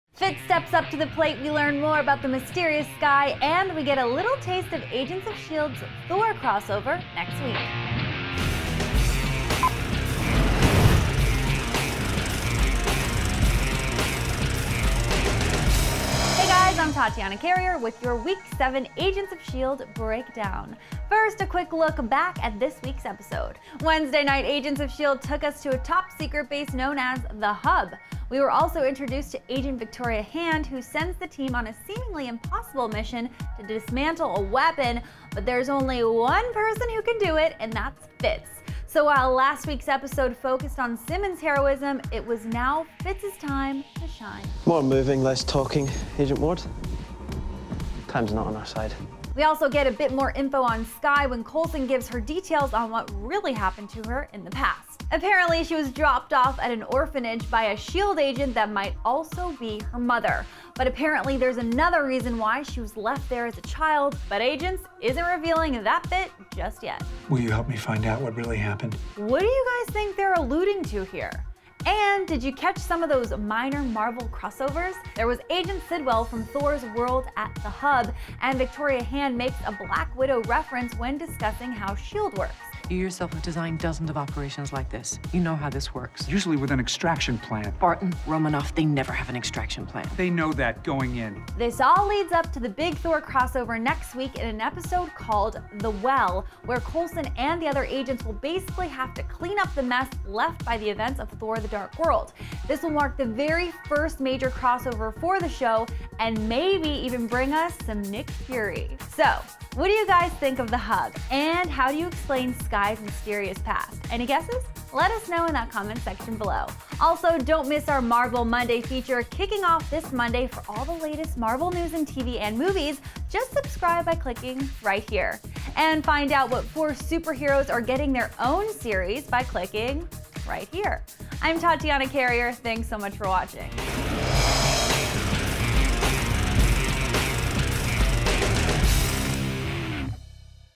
Agents Of SHIELD Episode 7 Teases Thor Crossover
Agents Of SHIELD Episode 7 recap and details on the big Thor The Dark World crossover.